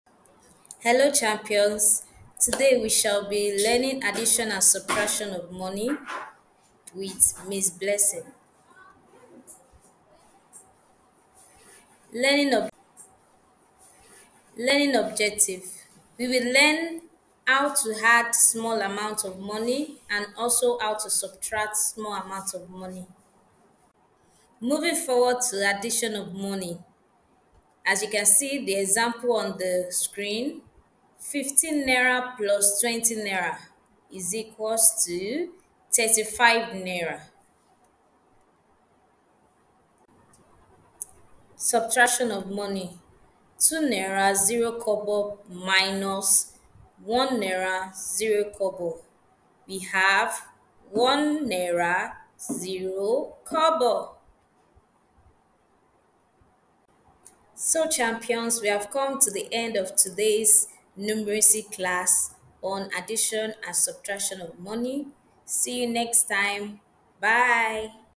Addition and Subtraction of Money (Part 1)
Learn how to add and subtract money accurately in real-life situations. Understand how to calculate totals, give correct change, and solve word problems involving currency. Improve financial literacy with practical examples.